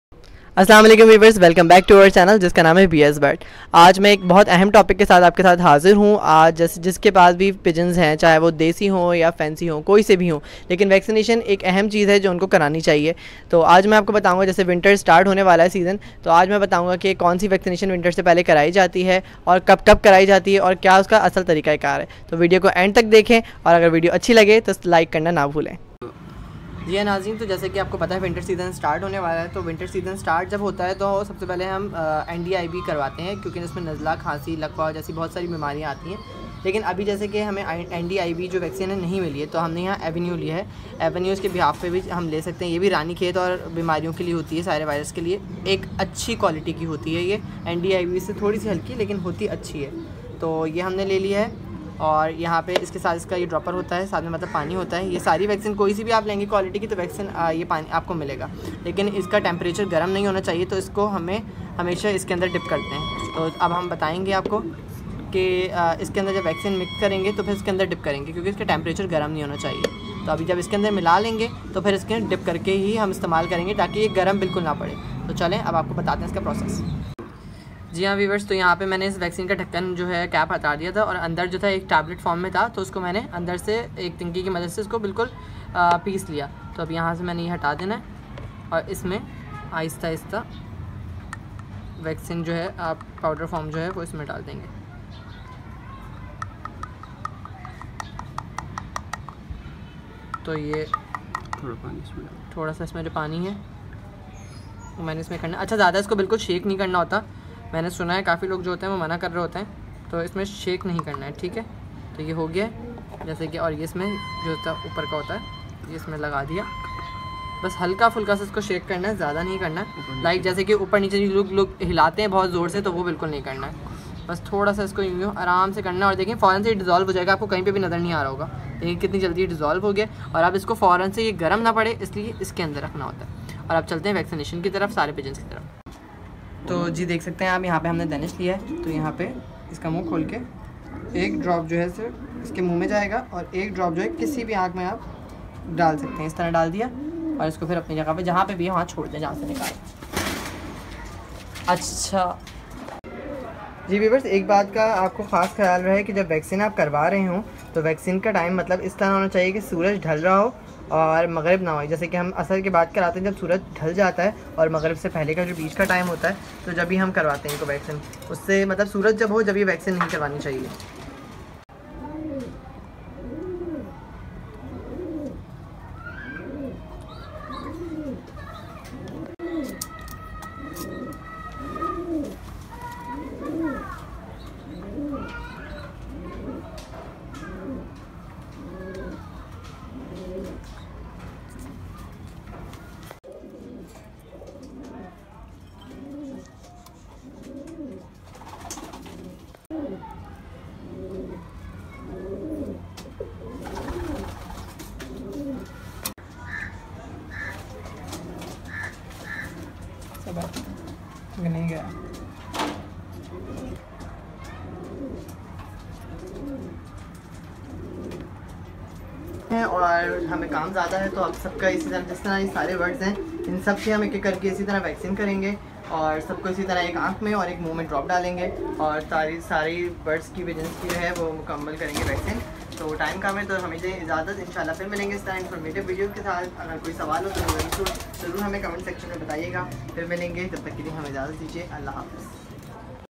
Pigeon ND Vaccination || Protect Your pigeon From Deadly Disease || Rani khet vaccine
All bird's vaccination especially fancy pigeon ND Vaccination Must Watch this video